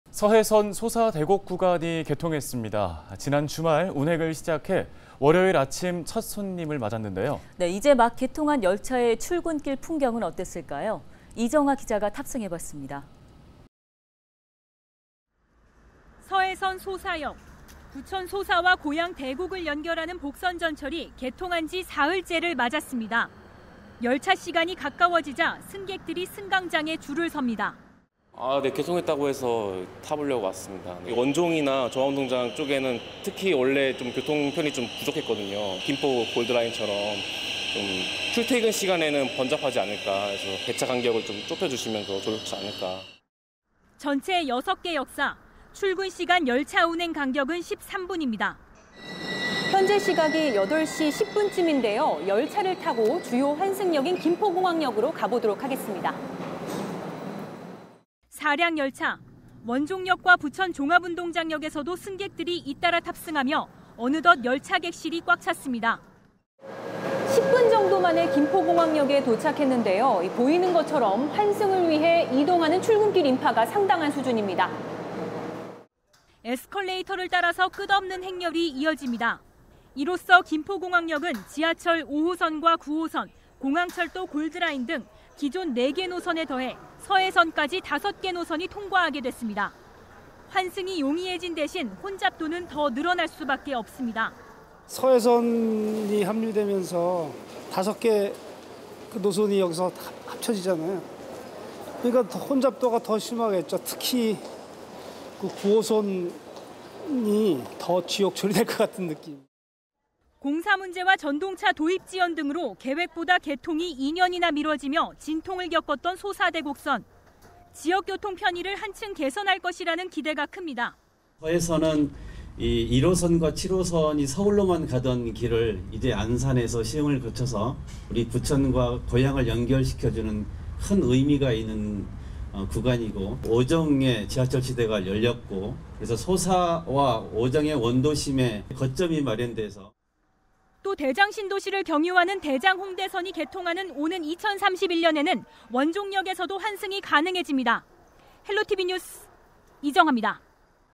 소사대곡선 '출근길 운행 첫날' 어땠나?
서해선 소사대곡 구간이 지난 주말 개통했습니다. 월요일 아침 첫 손님을 맞은 소사대곡 구간 출근길 풍경을 취재했습니다.